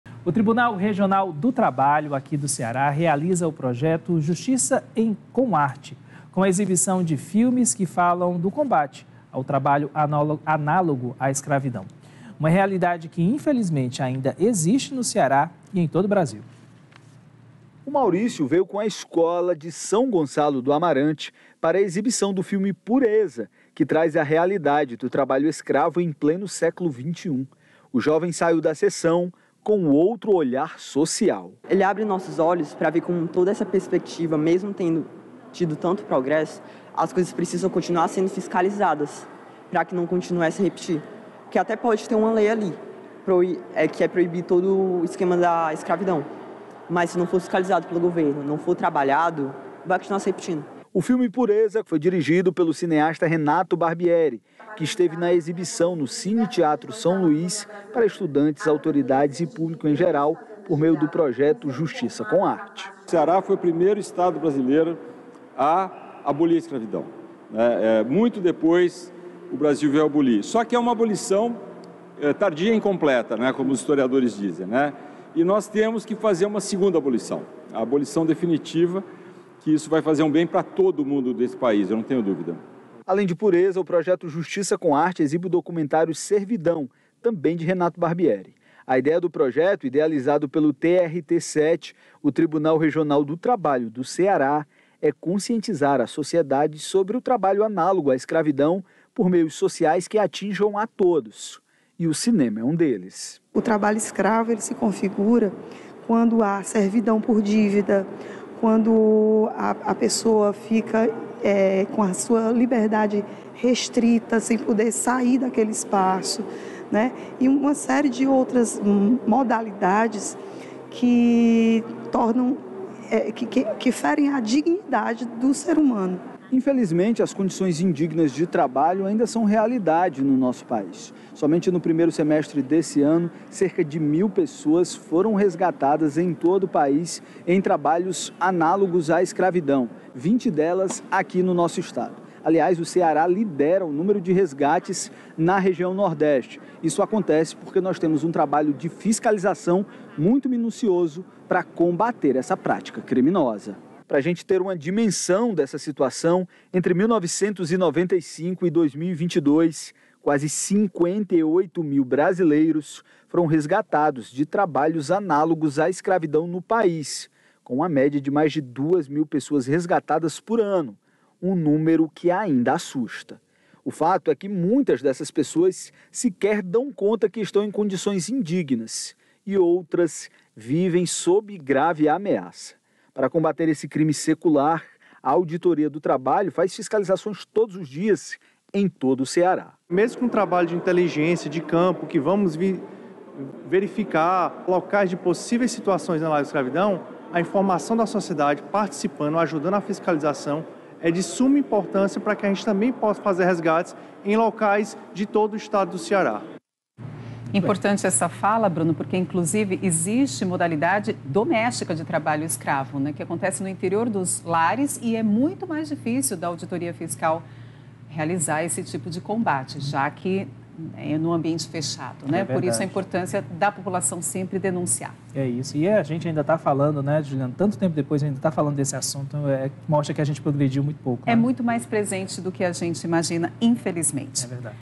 Programa Justiça com Arte - exibição do filme Pureza - matéria TV Ceará
O Programa Justiça com Arte do TRT-7, em parceria com o projeto Cine Educação, promoveu a exibição do filme “Pureza” no dia 13 de setembro, no Cineteatro São Luiz. O evento teve como debatedor o cineasta e diretor da obra, Renato Fortes Barbieri, que falou sobre trabalho escravo contemporâneo. Confira na matéria produzida pela TV Ceará.